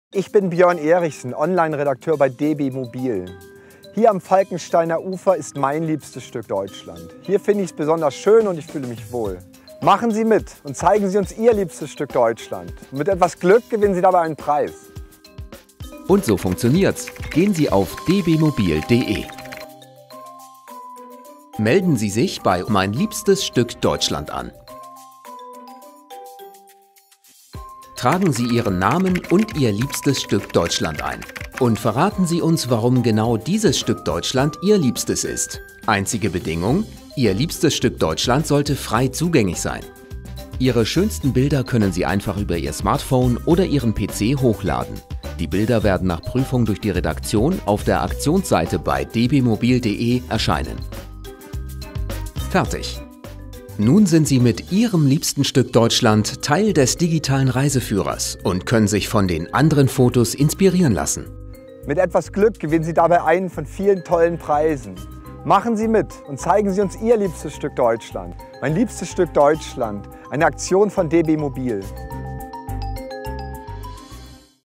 „Mein liebstes Stück Deutschland" - eine Aktion von DB mobil
Zeigen Sie uns Ihren liebsten Ort im Land! Bei der Aktion „Mein liebstes Stück Deutschland" können Sie gemeinsam mit dem Kundenmagazin DB mobil eine einzigartige Deutschlandkarte gestalten!

Laden Sie ein Foto Ihres Lieblingsortes hoch und verraten sie uns, was diesen so besonders macht. Es kann die romantische Parkbank sein, der belebte Platz, das windschiefe Gebäude oder ein einsamer Strand -- möglichst Orte von spezieller Schönheit, die aber nicht jeder kennt. Einzige Bedingung: Sie müssen öffentlich zugänglich und ihr Besuch kostenlos sein. Nach und nach entsteht so ein persönlicher Fotoreiseführer von den schönsten Ecken Deutschlands.

Mitmachen lohnt sich: DB mobil verlost jeden Monat unter allen Einsendern eine Reise. Außerdem werden ausgewählte Bilder aus der Sammlung im Kundenmagazin mobil abgedruckt.